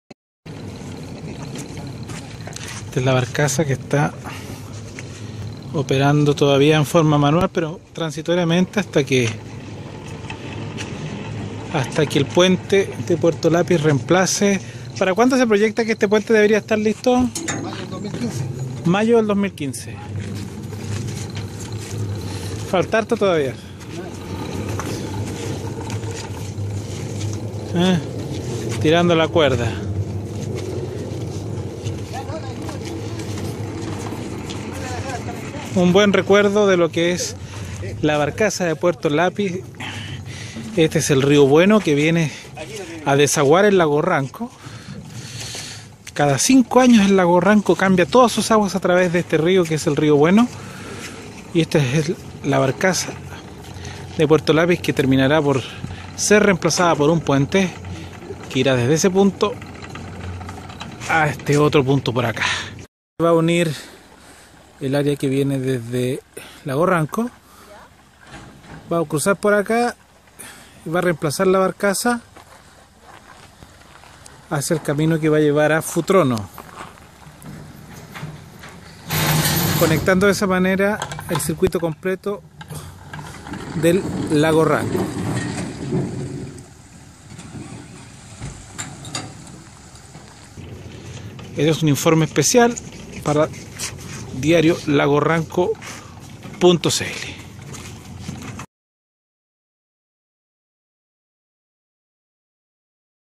DiarioLagoRanco.cl: Construcción del puente sector Puerto Lapi, red interlagos
Obras de construcción de camino y puentes Red Inter lagos. Más información